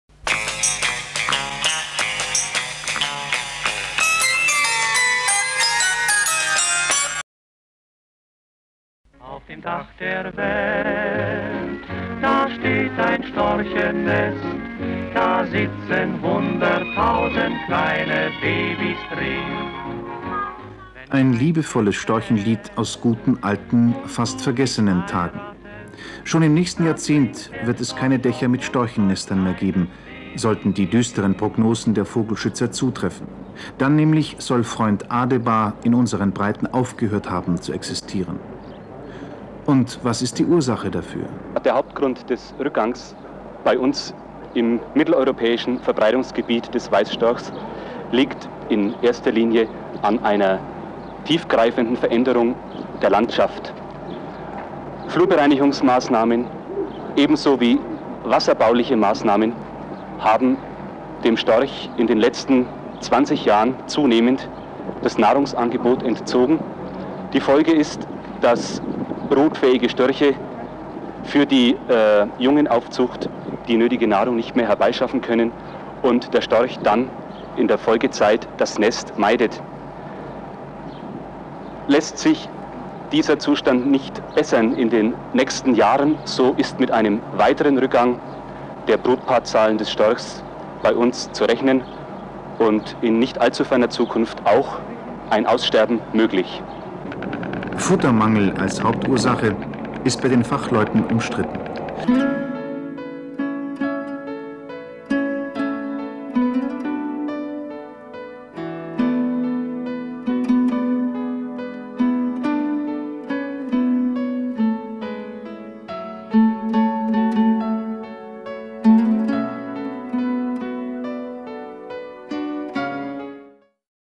Stoerche einst vom Aussterben bedroht
Der Storchenbestand war in den 8Oer Jahren durch negative Umwelteinflüsse stark bedroht. Der Tiefpunkt war 1988 mit nur 58 Brutpaaren. Hauptgrund war Nahrungs-mangel, hervorgerufen durch die Verringerung der Feuchtwiesen infolge der Inten-sivierung der Landwirtschaft. Es wurde ihm buchstäblich das Wasser abgegraben, denn durch die Trockenlegung ging die einstige Vielfalt der Nahrungskleintiere verloren. Landwirtschaftliche Maschinen, Kunstdünger und Flußbegradigungen verursachten eine große Beeinträchtigung seines Lebensraumes. Hochspannungs-leitungen verursachten 2/3 der Todesfälle, indem die Großvögel gegen die Masten prallten oder in den Starkstromleitungen endeten. Zu allem Überdruß wurde Freund Adebar auf seinem Weg in die südlichen Winterquartiere auch noch bejagt. Dank rechtzeitiger Schutzmassnahmen  hat sich der Storchenbestand bis 2o11 fast verdreifacht und vermehrt überwintern manche Brutpaare auch bei uns.